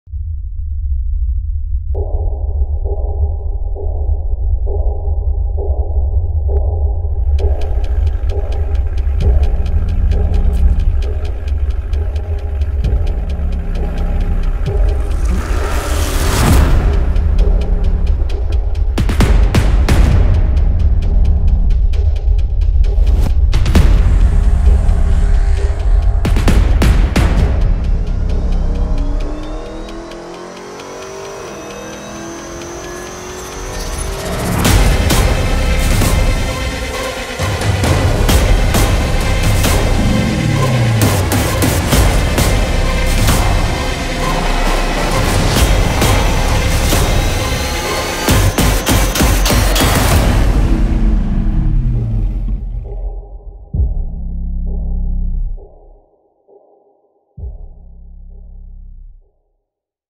Panthers Uniform Reveal: Black on Black ON BLACK
Minor change in the socks to bring major drip to the all-black uniforms.